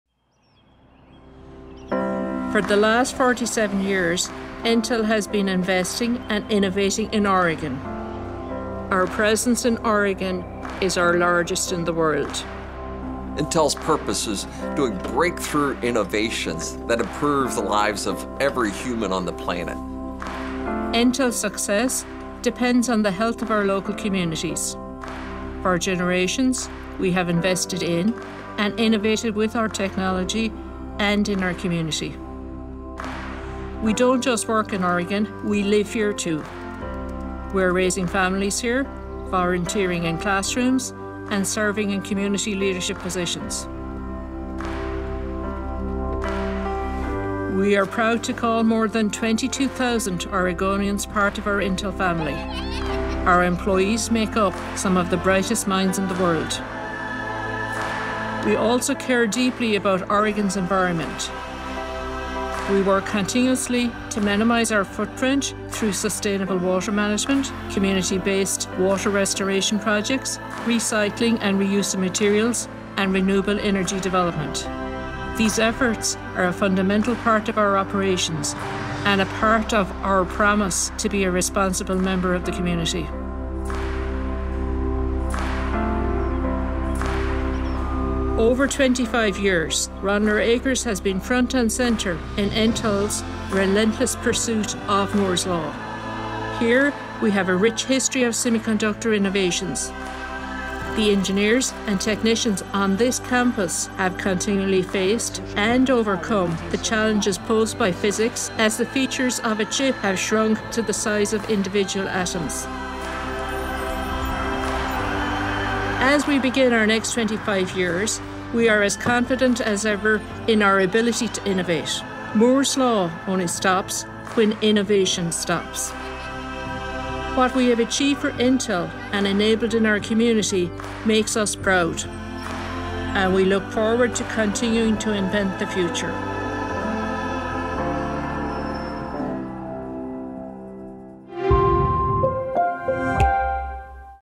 Intel's Role in Oregon
Intel has a history in the state of Oregon that stretches nearly 50 years. Intel's operations in the state represent its largest concentration of facilities and talent in the world, with close to 22,000 employees across four campuses in Hillsboro. The Mod3 expansion brings Intel’s total investment in Oregon to more than $52 billion. (Credit: Intel Corporation)